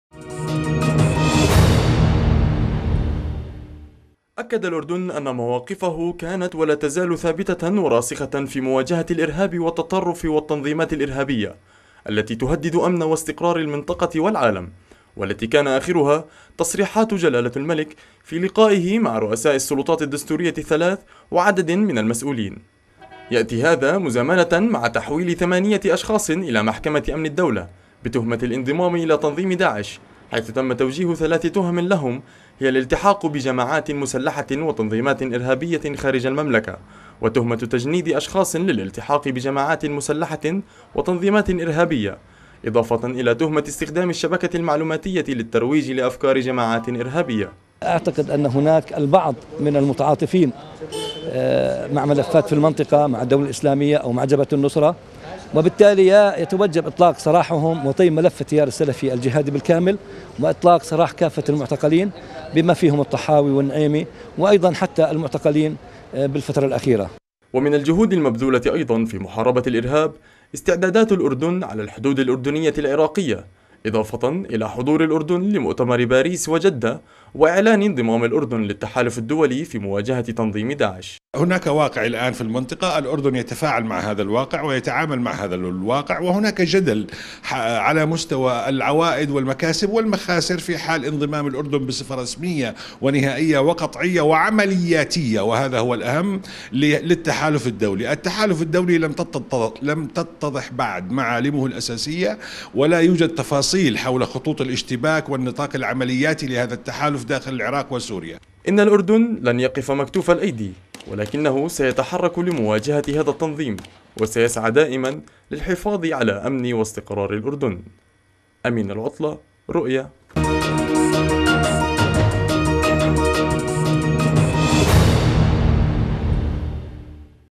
تحركات الأردن في مواجهة الإرهاب
انطلاقاً من موقف الأردن الثابت في مواجهة الإرهاب والتطرف، شارك الأردن عبر قنواته الدبلوماسية والسياسية مؤخراً في مؤتمري جدة وباريس المعنيين بتوحيد الجهود الإقليمية والدولية لمحاربة تنظيم الدولة الإسلامية داعش في المنطقة.

الأمر الذي انعكس أيضاً على الجبهة الداخلية وكان آخرها إحالة ثمانية متهمين إلى محكمة أمن الدولة للبدء بمحاكمتهم بتهم الانتماء لتنظيم داعش.